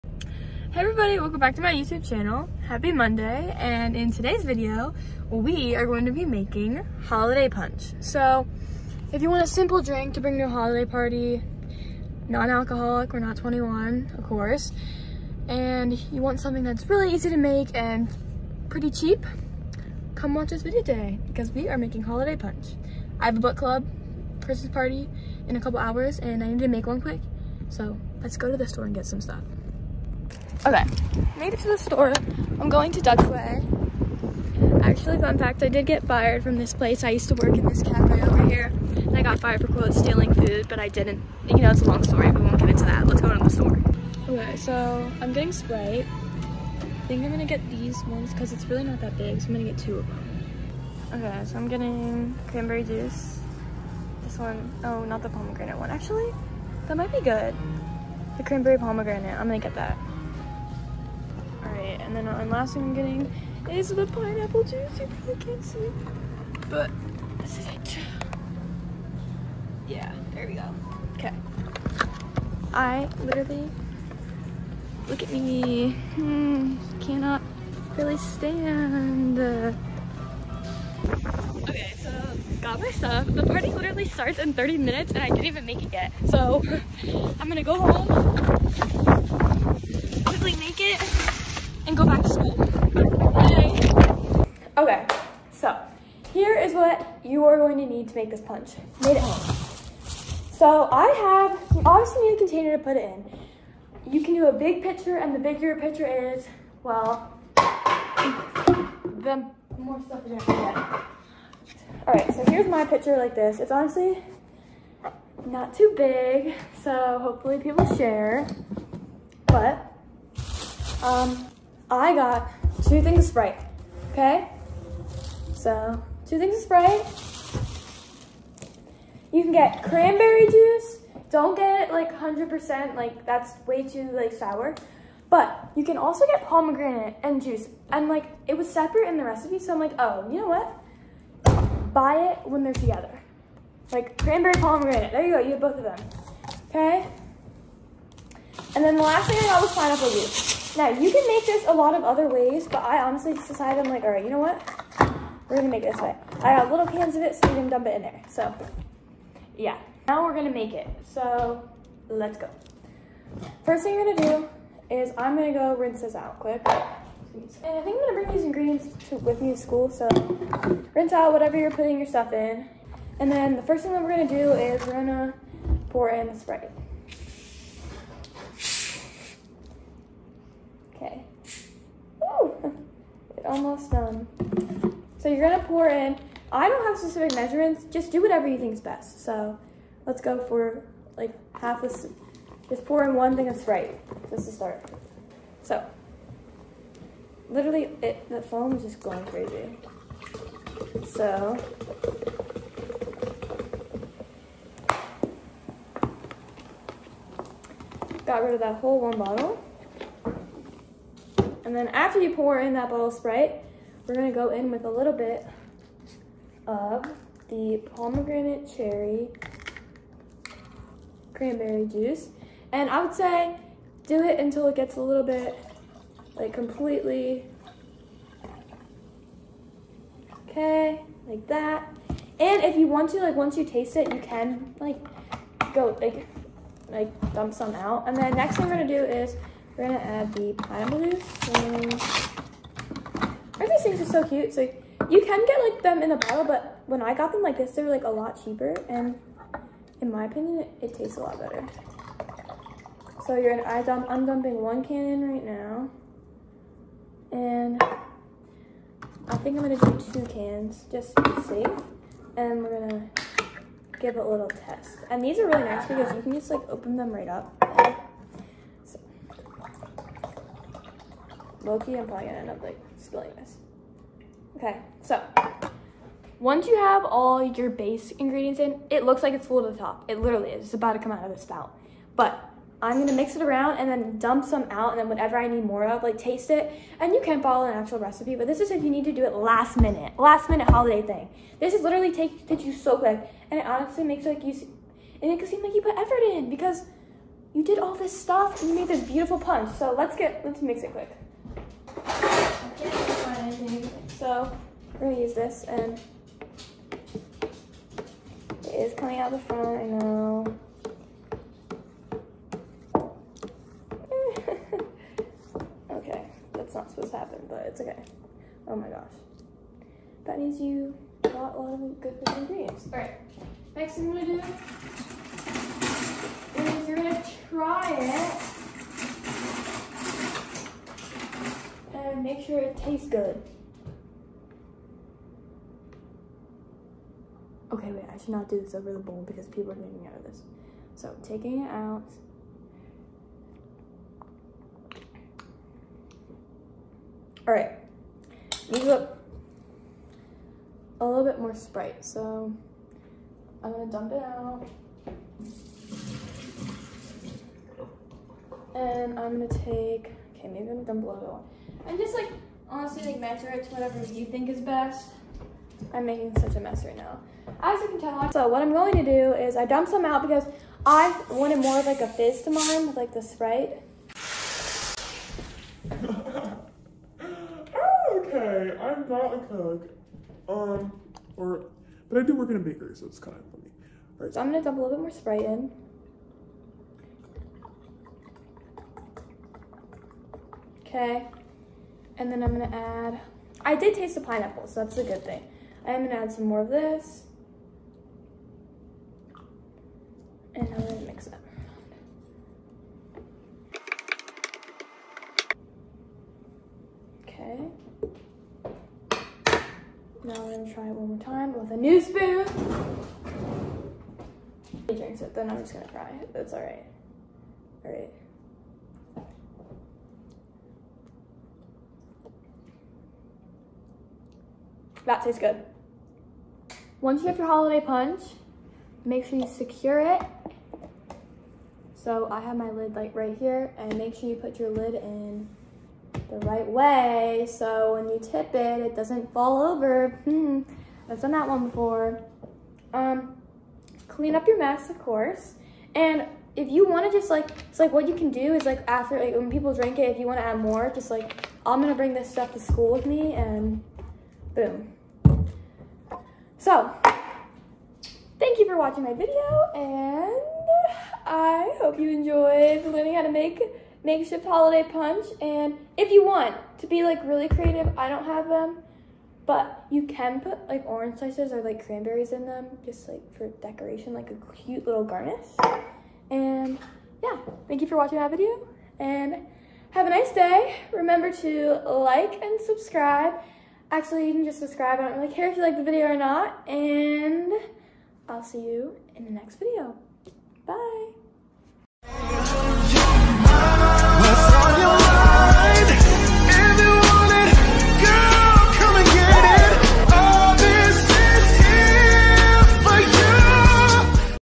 EASY HOLIDAY PUNCH RECIPE (less than 10 minutes)
Hey guys!! Happy Monday🤗
I’m today’s video we are making a very simple holiday punch recipe. If you ever need a quick drink to bring to a holiday party, try this out.
-serving size=1 pitcher
-Sprite (two medium sized bottles)
-one bottle of cranberry pomegranate juice
-two small cans of pineapple juice